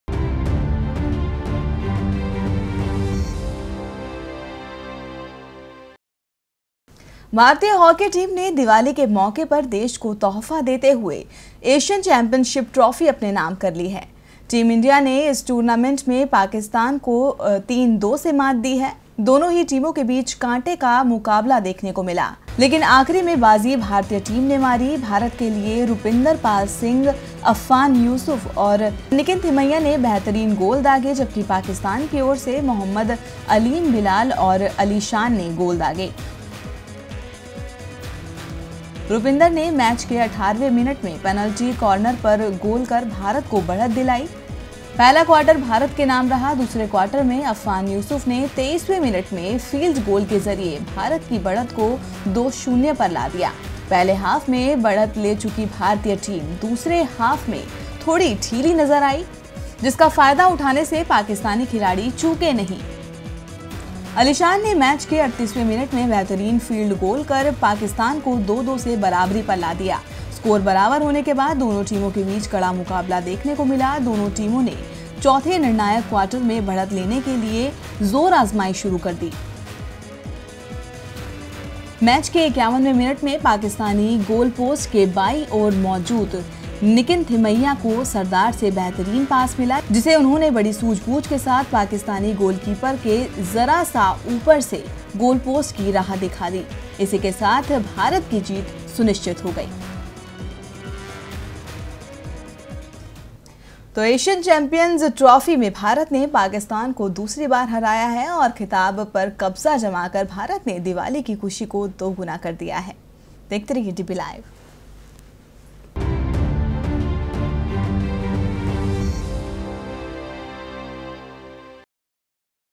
DBLIVE | 31 October 2016 | Hockey: India beat Pak to lift Asian Champions Trophy
भारतीय हॉकी टीम ने दिवाली के मौके पर देश को तोहफा देते हुए एशियन चैम्पियंस ट्रॉफी अपने नाम कर ली है। टीम इंडिया ने इस टूर्नामेंट में पाकिस्तान को 3-2 से मात दी है। दोनों ही टीमों के बीच कांटे का मुकाबला देखने को मिला लेकिन आखिरी में बाजी भारतीय टीम ने मारी। भारत के लिए रुपिंदर पाल सिंह, अफ्फान यूसुफ और निकिन थिमैया ने बेहतरीन गोल दागे, जबकि पाकिस्तान की ओर से मोहम्मद अलीम बिलाल और अली शान ने गोल दागे। रुपिंदर ने मैच के 18वें मिनट में पेनाल्टी कॉर्नर पर गोल कर भारत को बढ़त दिलाई। पहला क्वार्टर भारत के नाम रहा। दूसरे क्वार्टर में अफ्फान यूसुफ ने 23वें मिनट में फील्ड गोल के जरिए भारत की बढ़त को 2-0 पर ला दिया। पहले हाफ में बढ़त ले चुकी भारतीय टीम दूसरे हाफ में थोड़ी ढीली नजर आई, जिसका फायदा उठाने से पाकिस्तान खिलाड़ी चुके नहीं। अली शान ने मैच के 38वें मिनट में बेहतरीन फील्ड गोल कर पाकिस्तान को 2-2 से बराबरी पर ला दिया। स्कोर बराबर होने के बाद दोनों टीमों के बीच कड़ा मुकाबला देखने को मिला दोनों टीमों ने चौथे निर्णायक क्वार्टर में बढ़त लेने के लिए जोरआजमाइश शुरू कर दिया। मैच के 51वें मिनट में पाकिस्तानी गोलपोस्ट के बाईं ओर मौजूद निकिन थिमैया को सरदार से बेहतरीन पास मिला, जिसे उन्होंने बड़ी सूझबूझ के साथ पाकिस्तानी गोलकीपर के जरा सा ऊपर से गोलपोस्ट की राह दिखा दी, इसी के साथ भारत की जीत सुनिश्चित हो गई।